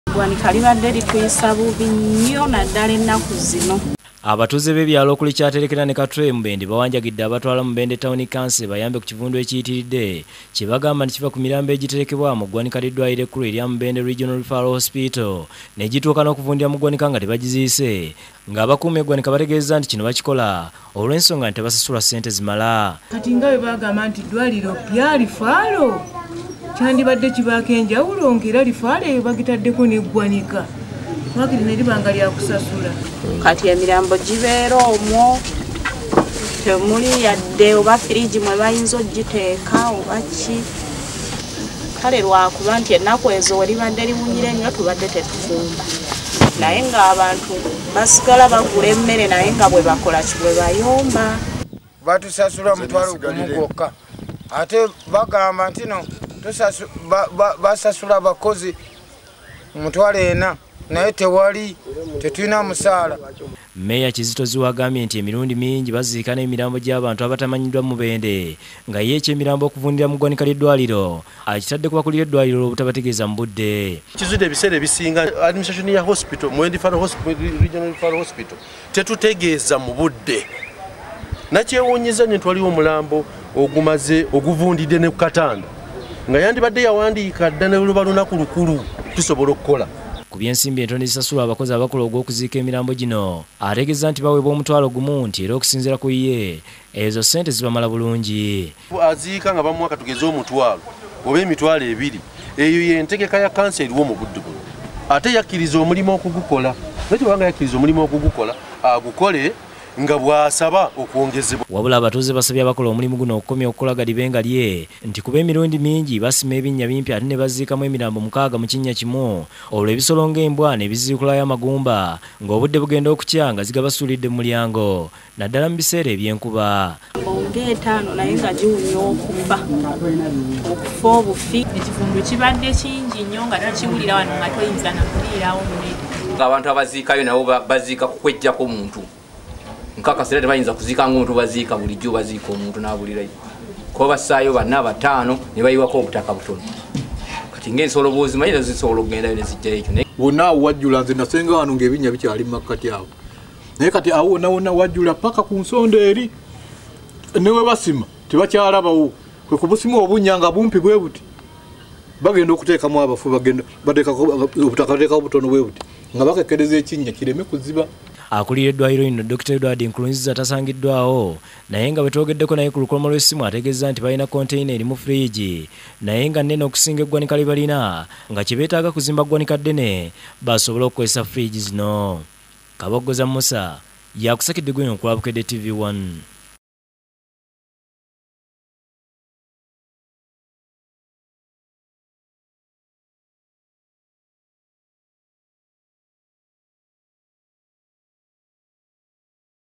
Abantu balaajanidde kkanso ku mirambo
'Ekivundu ekiva mu ggwanika kitutta mututaase'